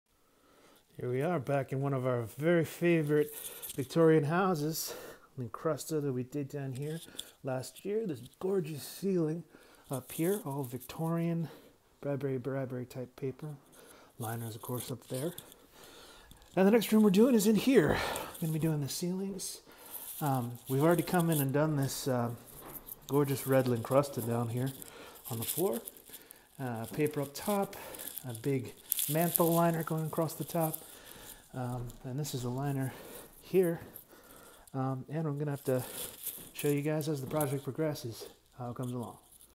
Bradbury & Bradbury Victorian Wallpaper
Next wallpaper project is at one of our favorite Victorian homes on Capitol Hill in DC. The wallpaper is Bradbury & Bradbury who specialize in hand printed 19th & 20th century wallpapers. The paper designer is William Morris.